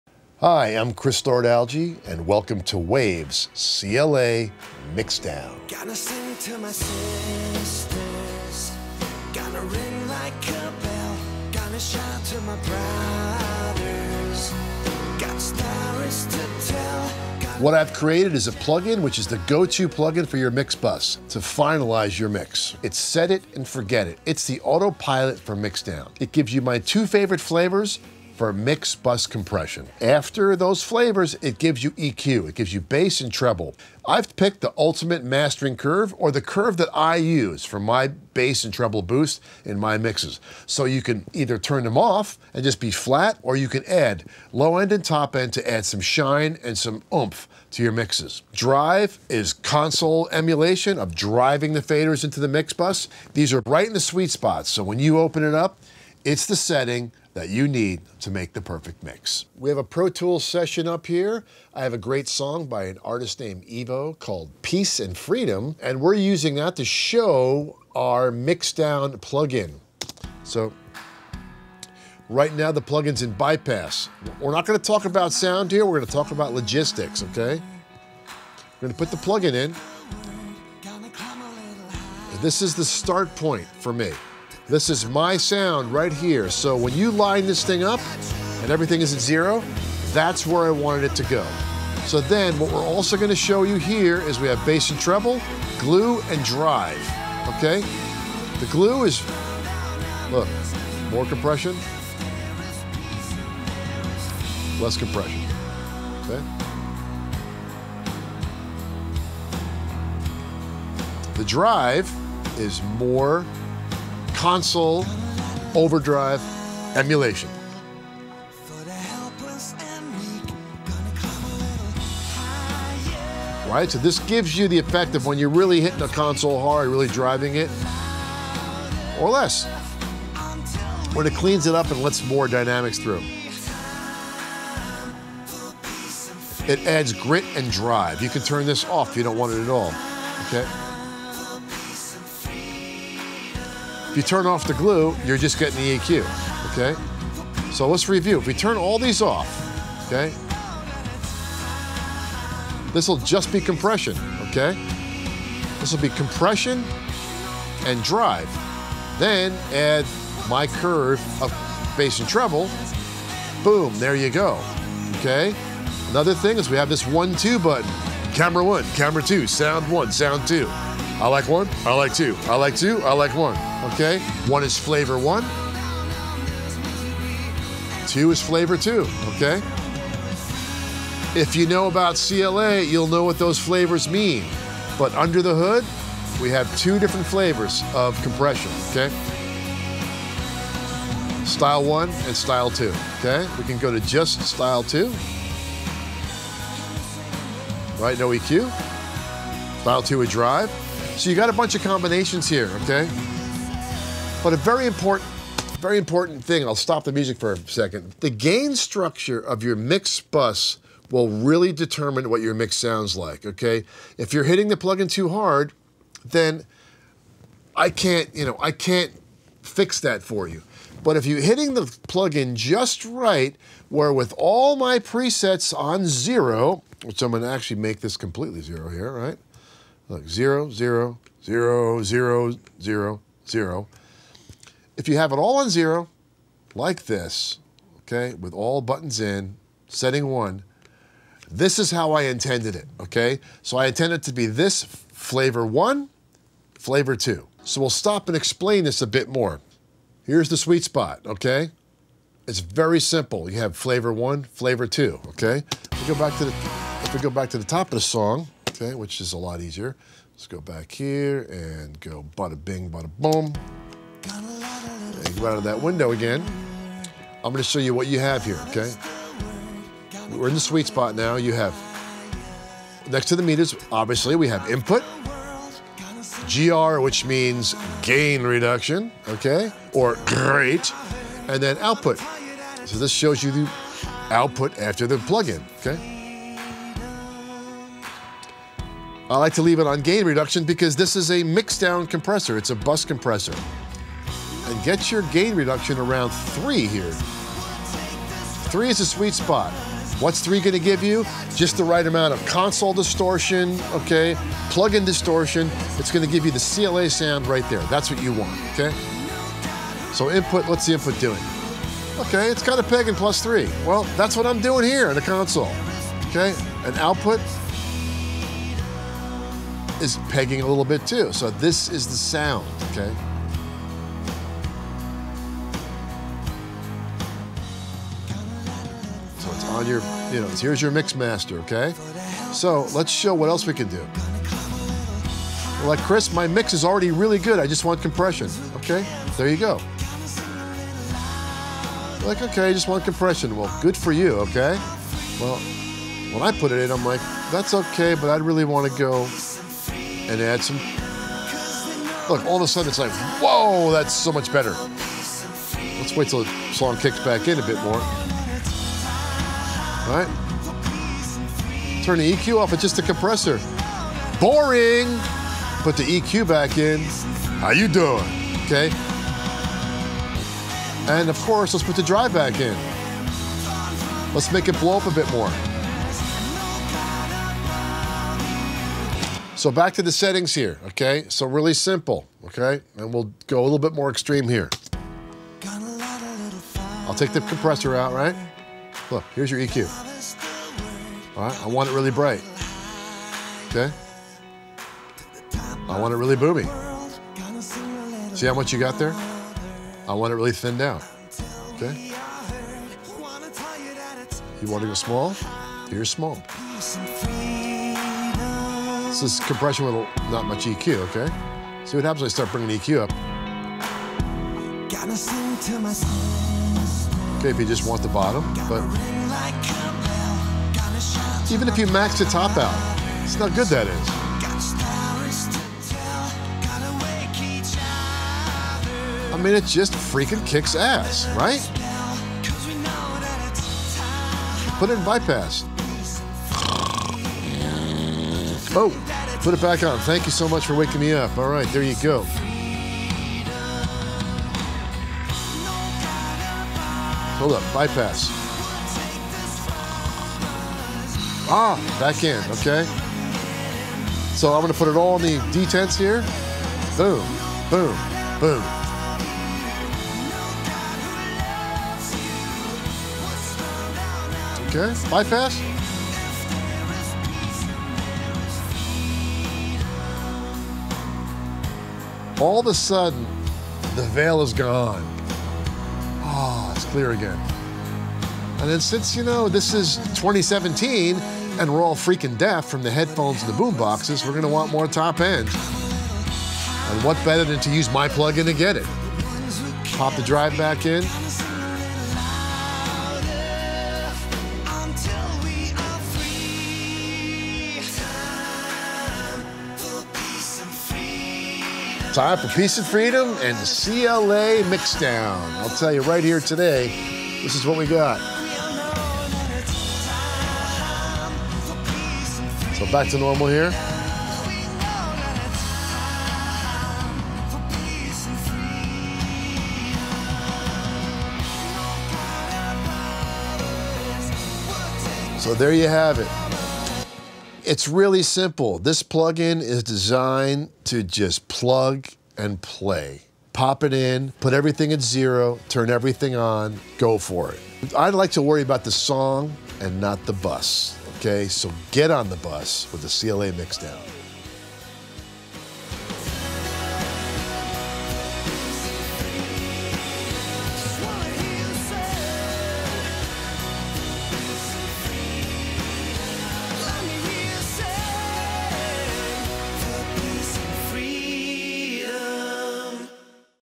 CLA MixDown – Plugin Overview with Chris Lord-Alge
Get inside tips from Grammy-winning mixer Chris Lord-Alge (Green Day, Bruce Springsteen, Keith Urban, Muse) on how to finalize your mixes with his signature mix buss plugin.

In this video Chris shows you how to:

- Combine the plugin’s mix buss compression, EQ and console overdrive options
- Properly set gain reduction with CLA MixDown
- Set CLA MixDown to the ‘sweet spot’ that captures most closely how Chris himself normally processes his mix buss, vs. driving the plugin harder when needed